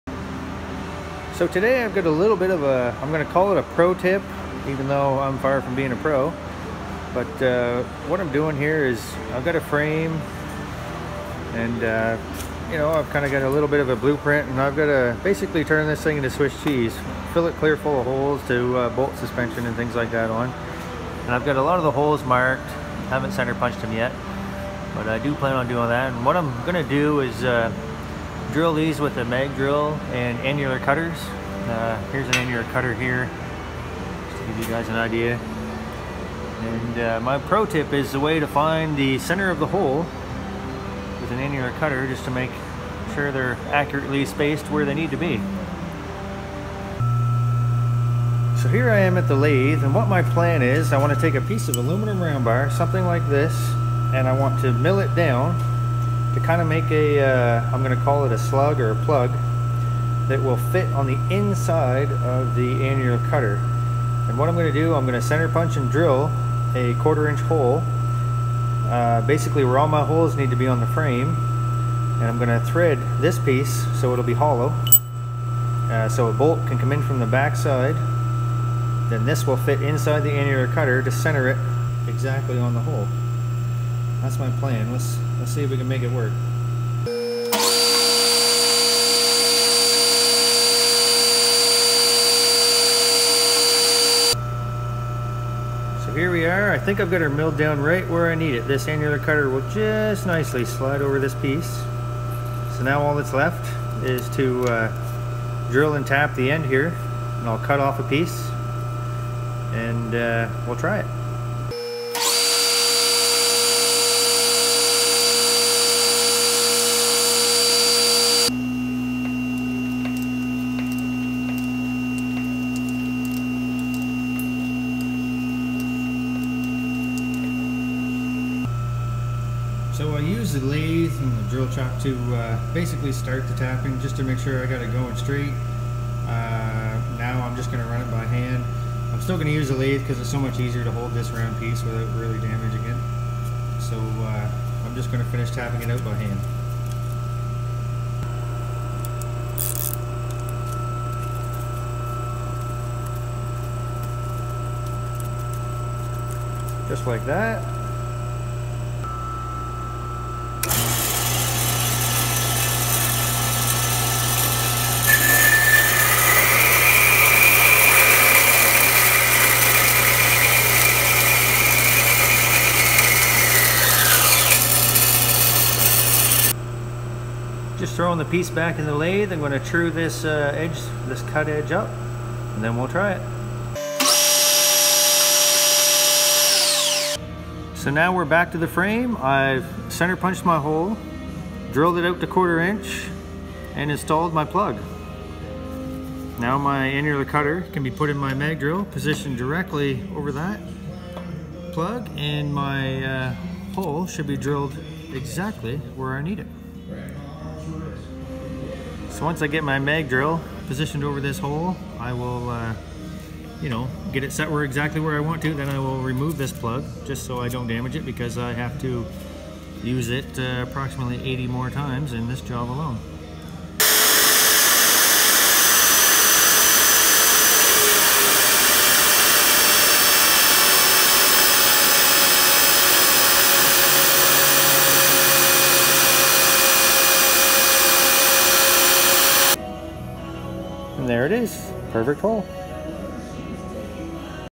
TIP/TRICK- Drilling with annular bits.
This is a tip/trick to aid in drilling holes exactly where they need to be, using annular cutter bits. Just an idea I had. And there may be a similar product that already exists. I never researched it and couldn’t wait for it either. Thanks